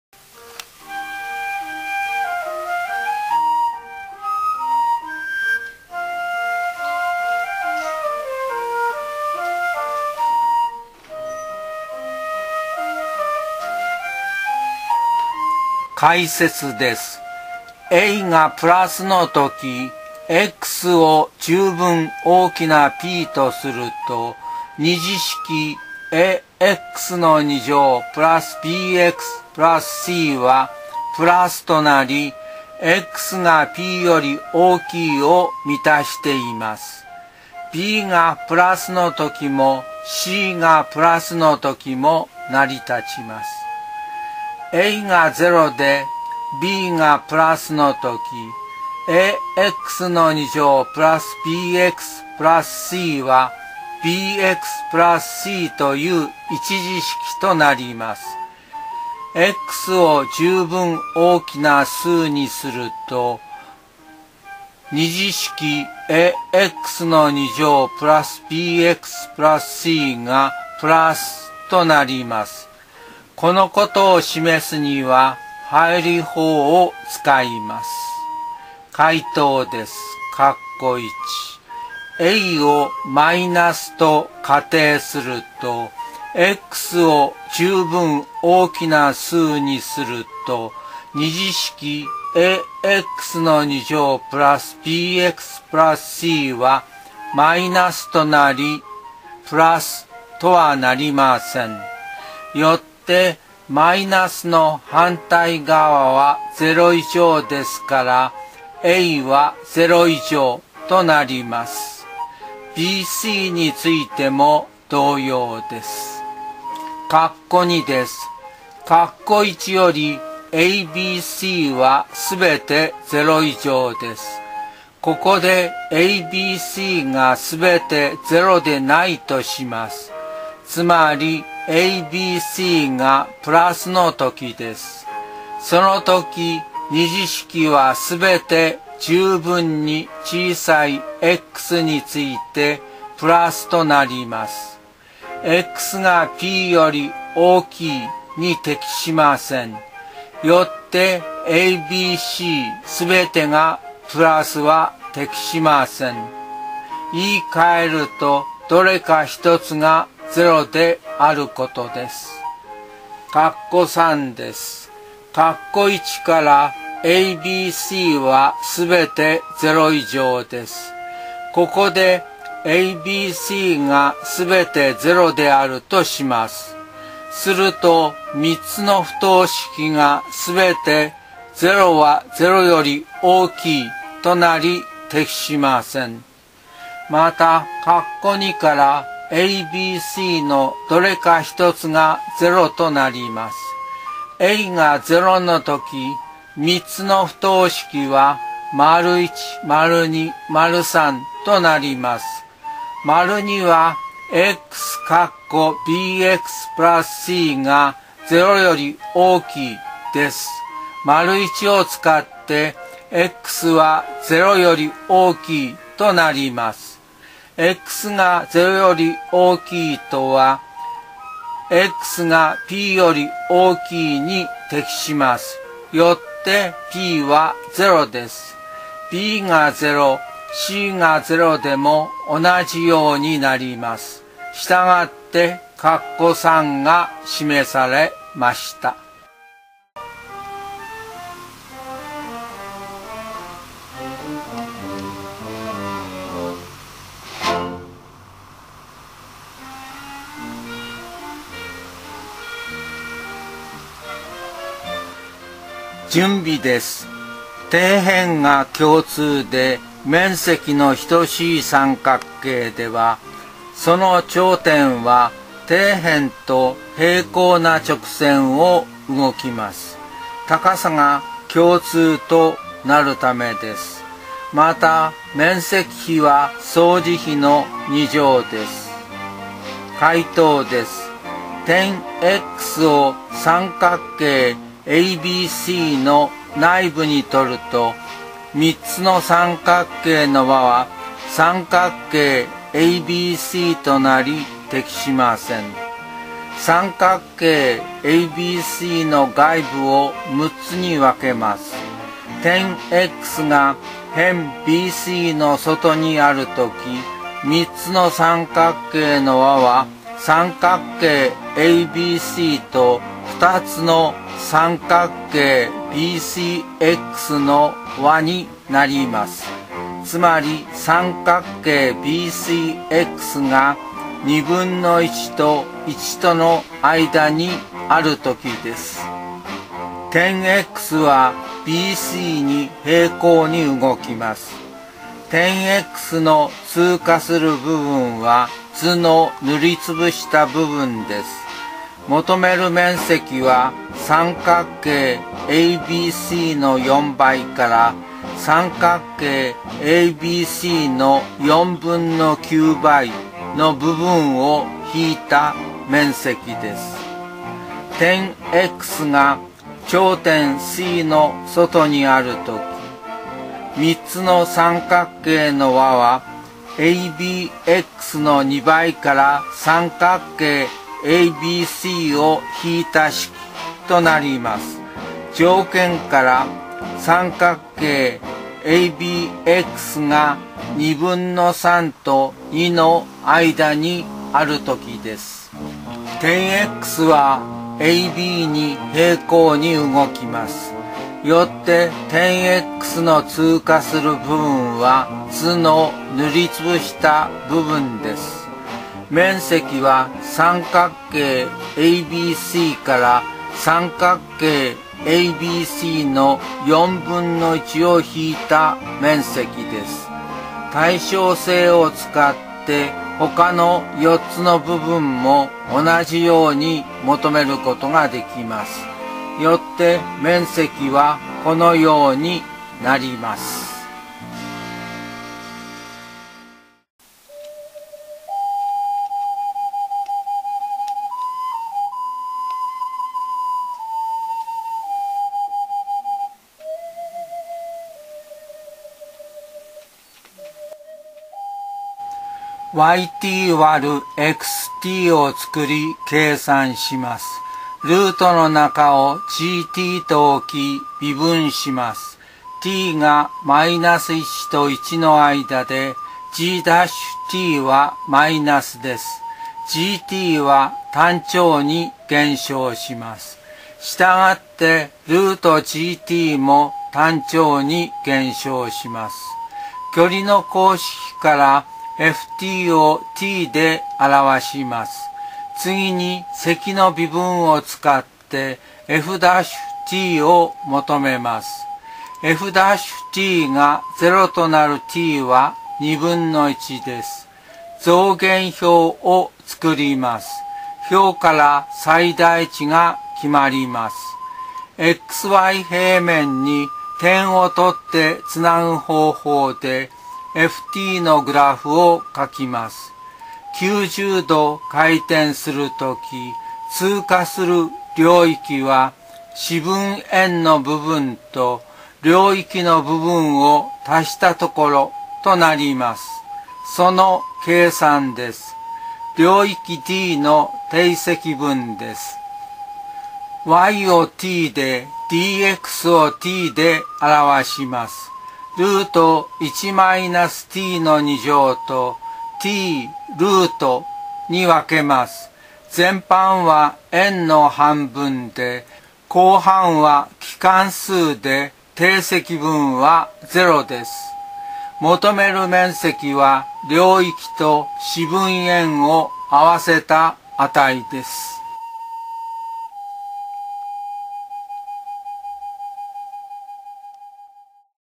東京大学２次試験問題2020年・数学理系第１問～第３問
「早い、やさしい、うまい」という方針で解いてみました。